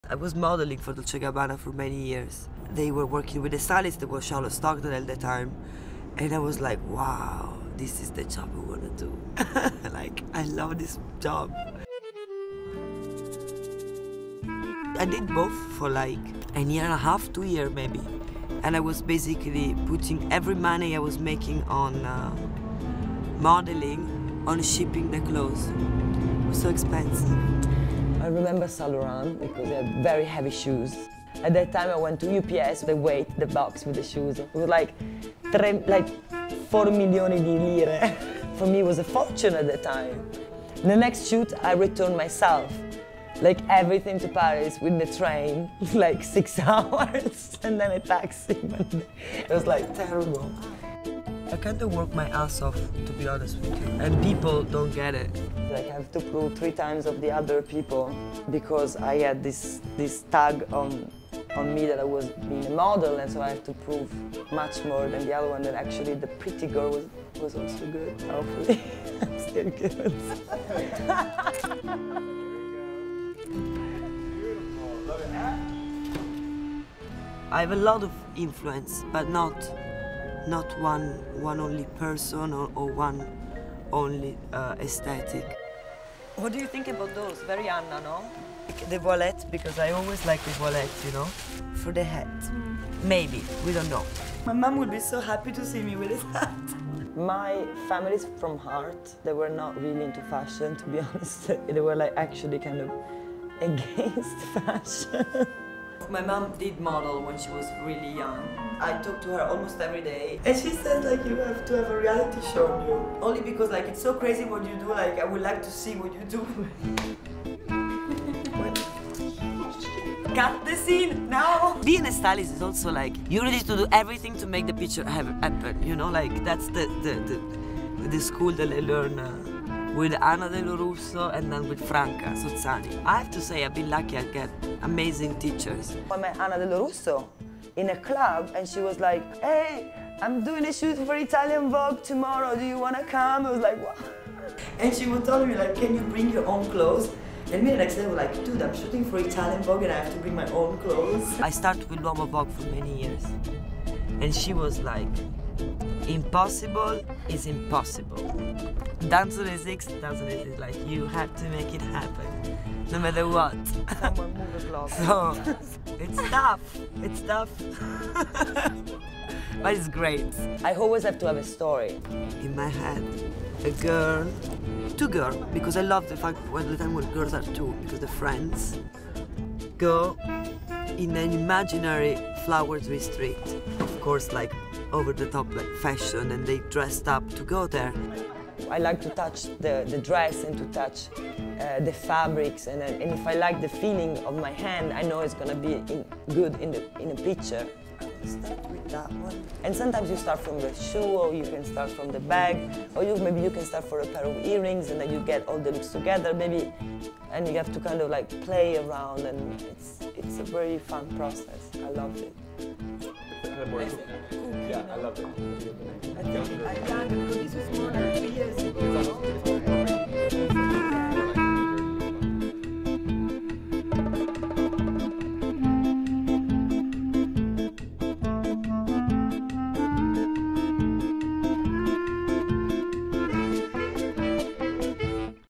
Giovanna Battaglia
See W Contributing Fashion Editor Giovanna Battaglia on the job in the first installment of our new video series featuring behind-the-scenes glimpses of fashion's most intriguing personalities.